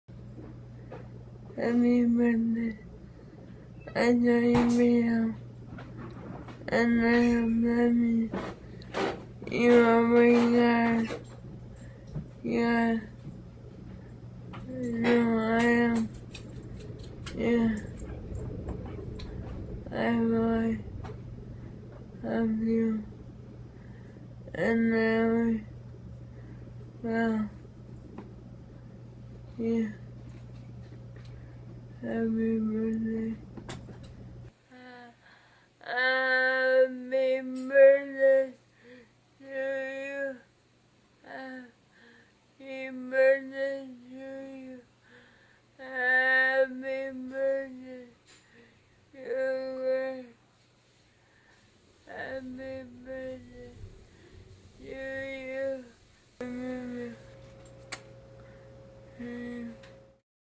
Happy Birthday Chris Medina, Love Juliana
Juliana wishing Chris Medina a happy birthday while he is on tour.